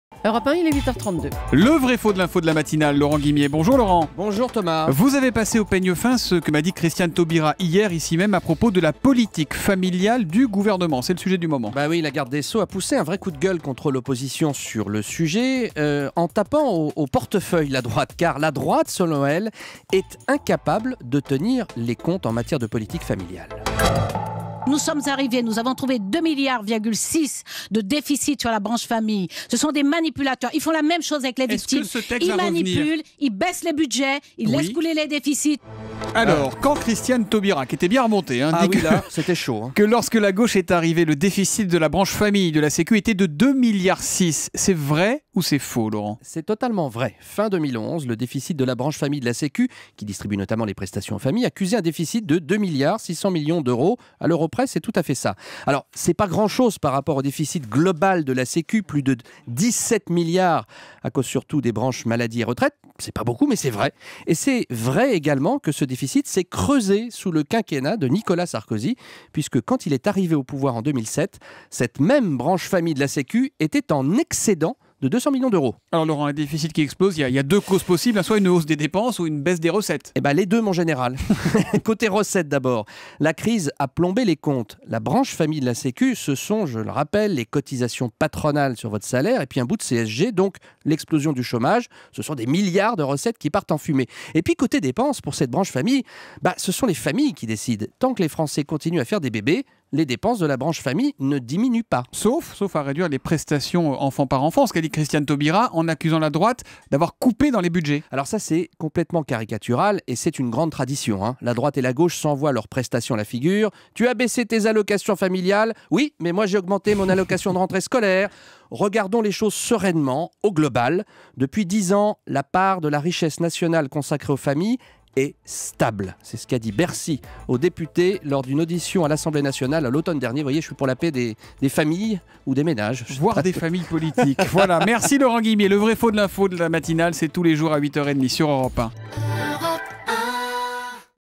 Christiane Taubira pousse un coup de gueule
Selon elle, la droite est incapable de tenir les comptes en matière de politique familiale.